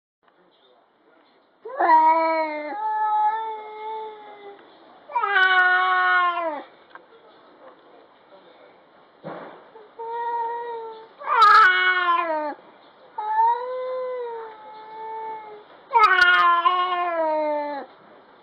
ルーさん・ケンカ？する
庭にやってきたノラ猫さん。
うーうー文句言ってたら、飛び掛って
怒られちゃいました。